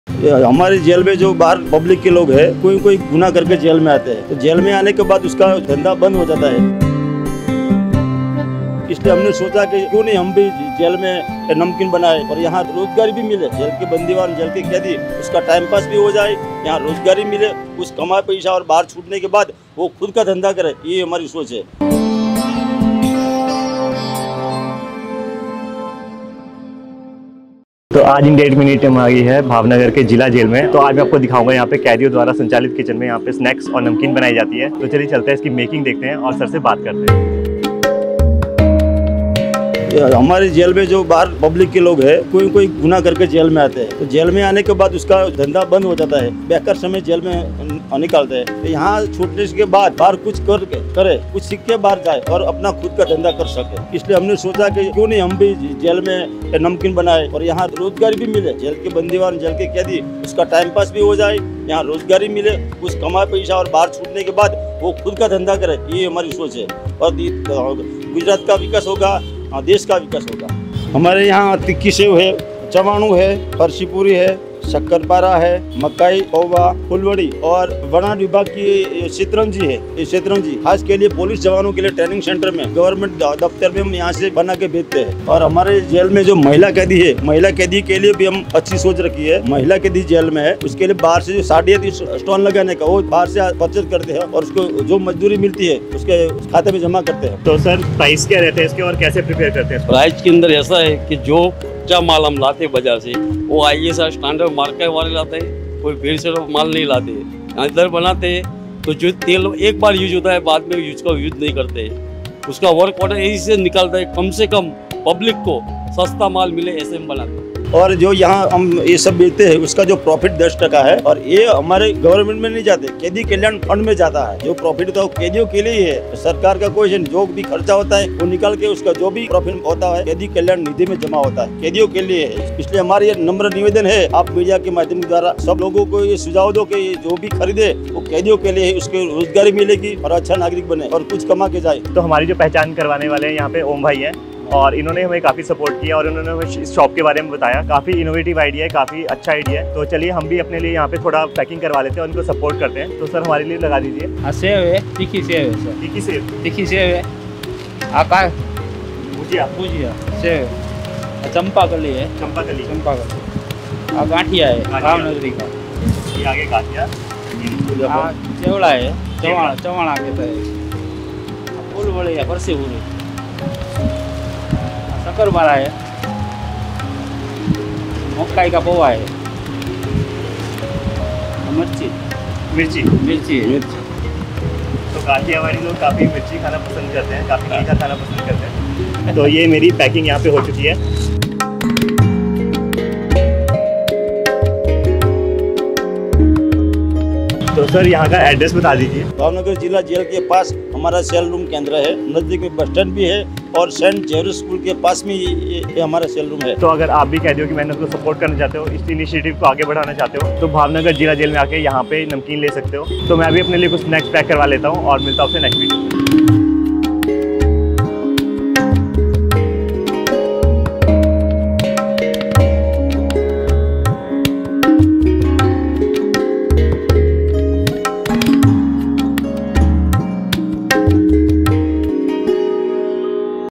Bhavnagar Jail Prisoner Making Snacks l भावनगर के जेल में कैदियों दुवारा बनाये जाती है गुजराती नमकीन
Address: Devbagh, Bhavnagar, Gujarat 364001
Social Media Platforms  @INDIA EAT MANIA